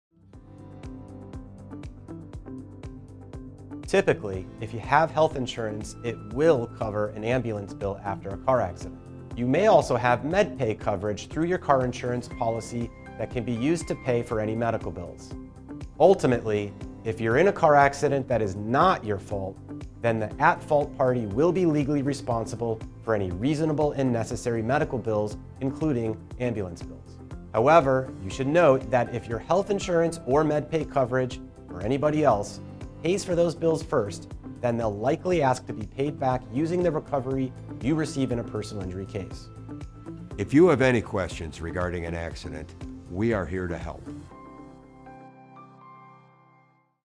Does Insurance Cover An Ambulance After A Car Accident? | The Barnes Firm Injury Attorneys
Does insurance cover the cost of an ambulance after a car accident? The Barnes Firm's top car accident lawyer in San Diego provides the answer:

If you have health insurance, or med-pay coverage through your car insurance policy, the cost of an ambulance is typically covered. However, this gets more complicated when you're injured due to someone else's negligence because it's then up to their auto insurance policy to pay for your medical costs. Many times, the insurance companies may refuse to cover it.

That's where an experienced car accident attorney can come in and make sure all the bills that should be paid by insurance are being paid.

Additionally, San Diego car accident lawyer Christian Oliver says that if you do have ambulance costs or other medical bills covered through health insurance AND you receive a personal injury settlement, your health insurance (or other provider) will likely request reimbursement with what's called a lien.

If you have any questions about car crashes, injuries, your rights, or other queries about the law, The Barnes Firm is here to help.